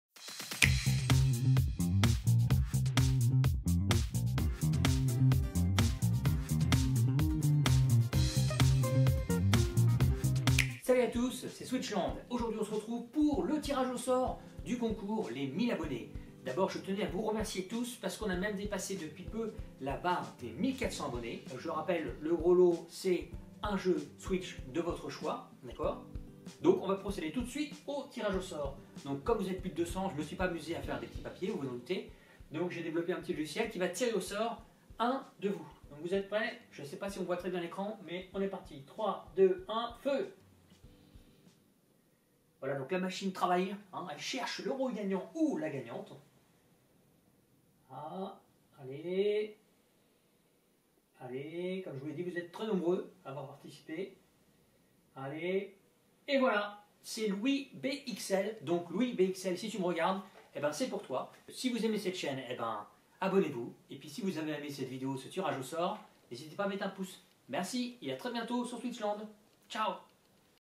Concours 1000 abonnés, le résultat !!
Pour remercier tous mes abonnés, j' offre un jeu switch a l'un d'entre eux !!